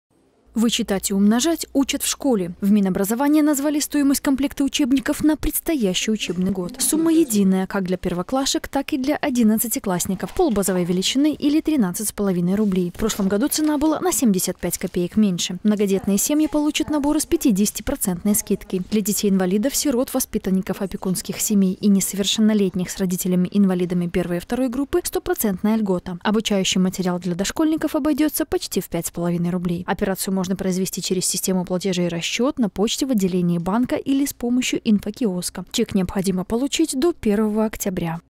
Кто получит школьные учебники и пособия бесплатно
Вычитать и умножать учат в школе. В Минобразования назвали стоимость комплекта учебников на предстоящий учебный год.